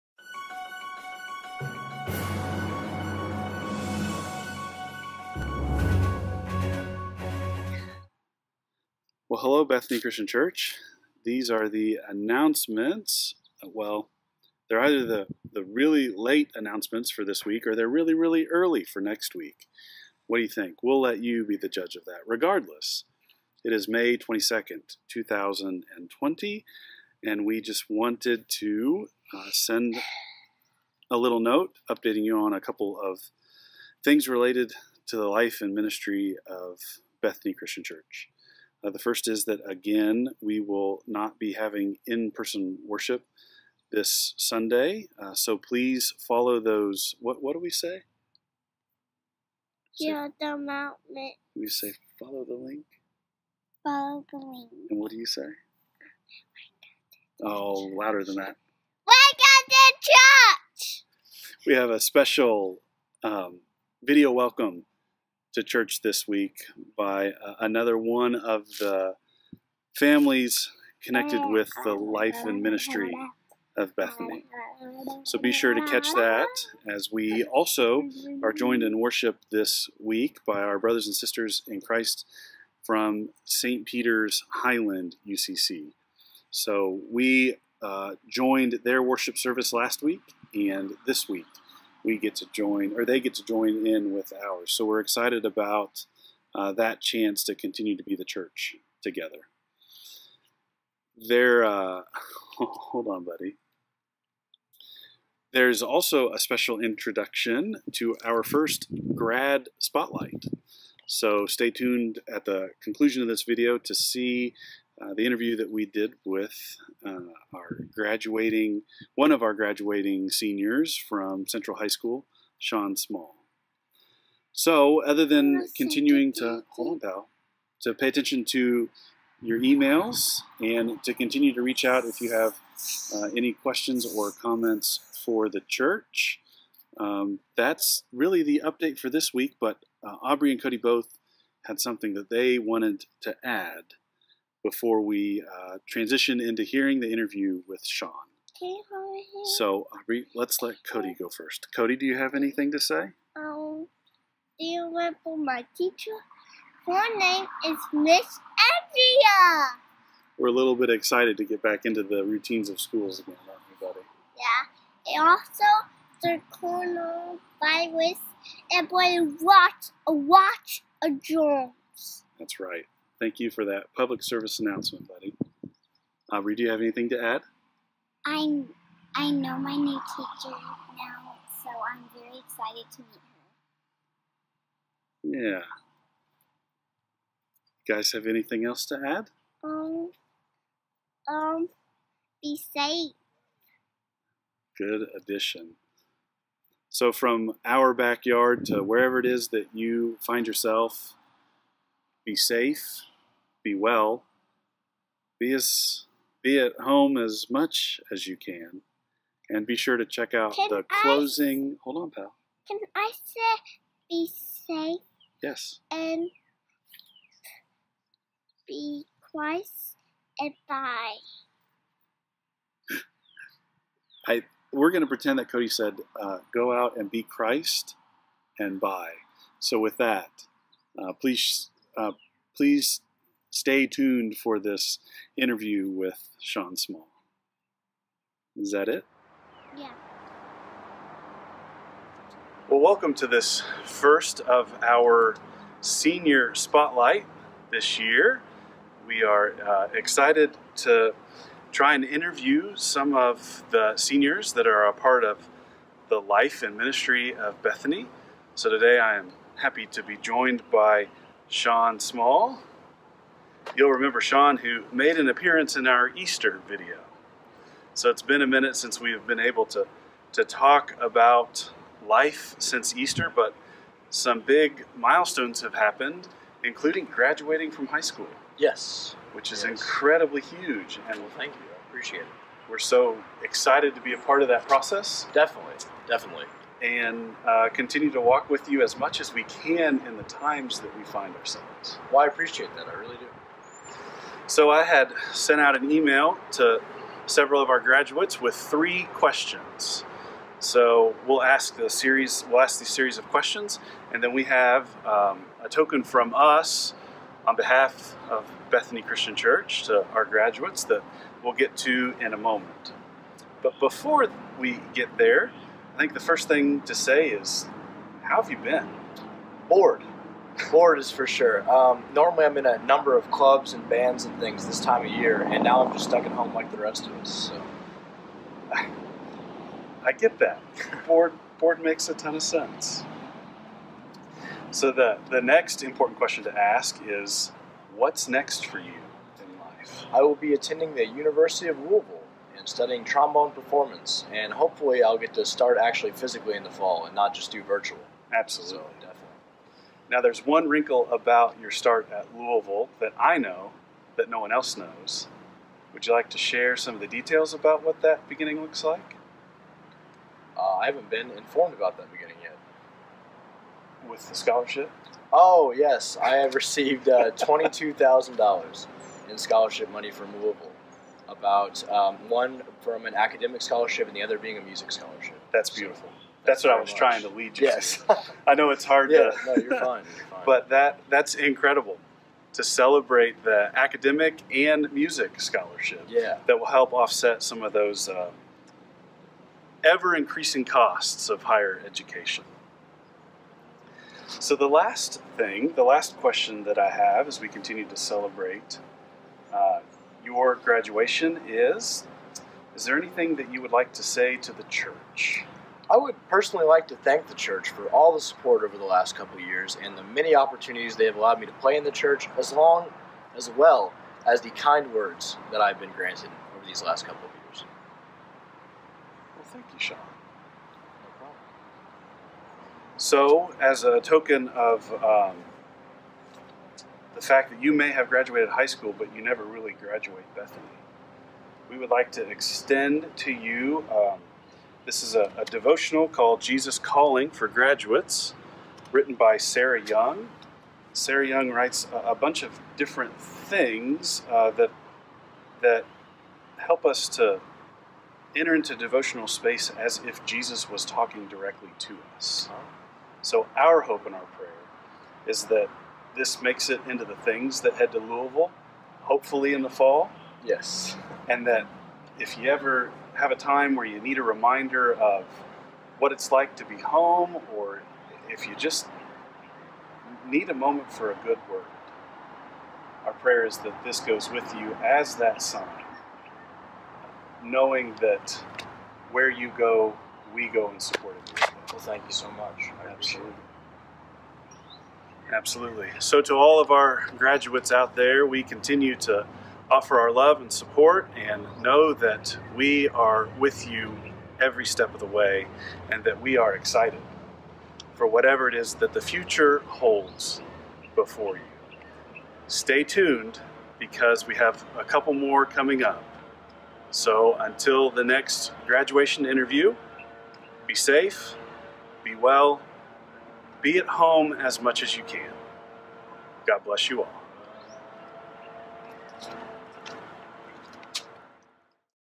5.22.20 Announcements
Stay tunned through the end for the first of our special 2020 graduation interviews!

To support the mission and vision of Bethany, please follow this